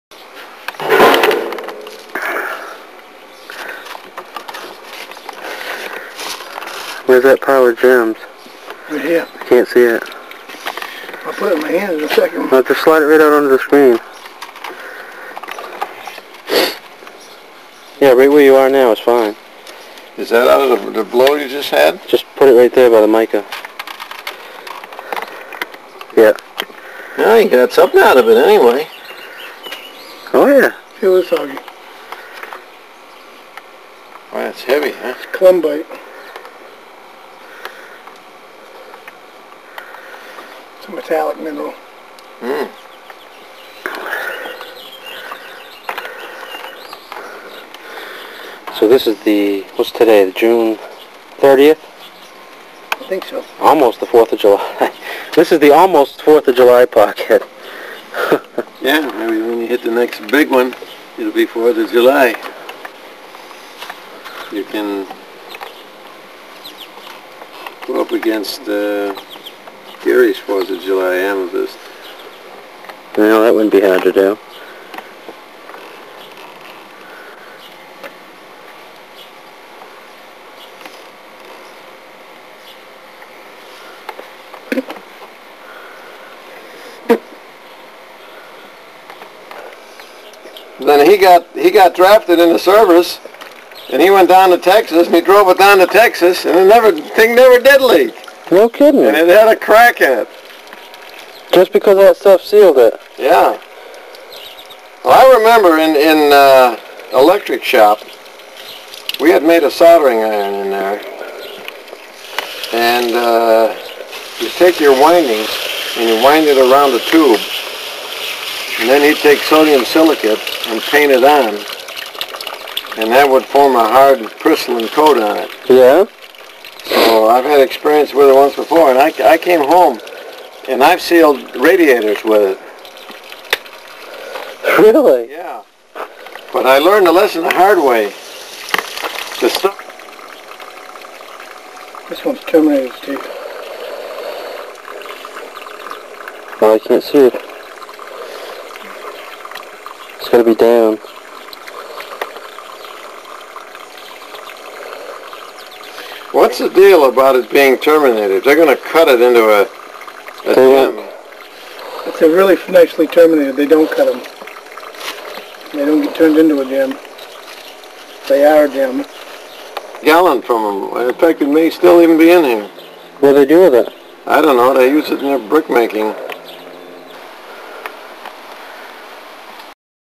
Sifters 1997 Fun
This is the fun part; Randy Stone and I wash the pocket contents. Randy worked the pegmatite with me from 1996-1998. We sifted through tons of buckets of pocket material extracted. Some of the best was saved for tourmaline sand to try and sell to fund raise and the rest was dumped over in the trailer park. For years until they dumped dirt over it the spread out piles still yielded gem pieces that were not washed off well enough to find.Visit our